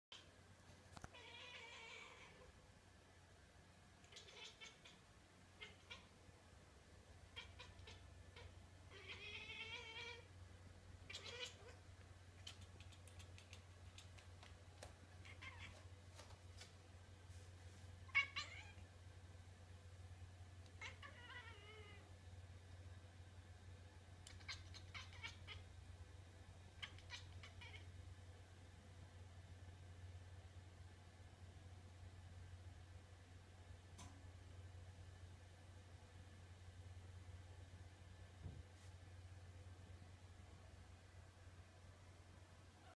Siamese cat speaks foreign language
He's trying to imitate bird voice